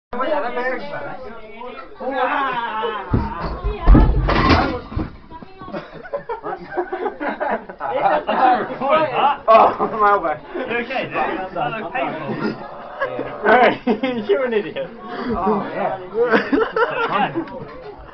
Going down t'stairs
Me falling down some stairs